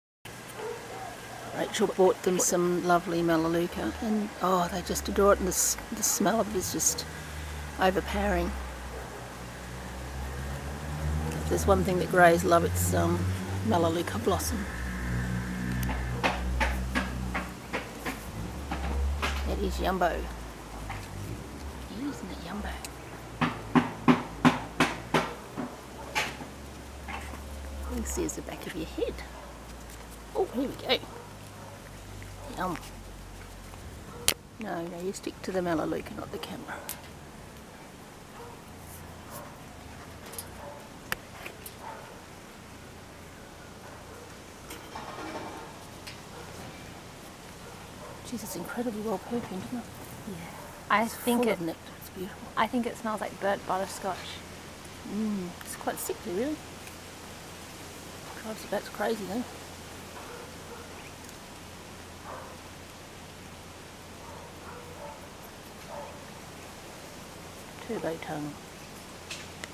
Grey-headed flying-fox feasting on blossom
Flying-foxes feast on all manner of things like leaves, flowers, bark and fruit but their absolute favourite is nectar and pollen.
Thousands of years of evolution has produced the perfect nocturnal pollinator and an important symbiotic relationship with Australian forests has been forged between bats and forests.
Birds and bees perform the day shift but flying-foxes are our only long distance nocturnal pollinators that help to keep our forests healthy and diverse ensuring the health and longevity of important native species.
Flying-foxes have long raspy tongues that lap up nectar and in the process, their faces become covered in pollen.
Off they fly to the next flowering tree before burying their muzzle in the blossom thus ensuring cross pollination.
With all four species in rapid decline, it is not only bats that face an uncertain future for without our pollinators, forests will sicken and die.
Bats. protect them or lose them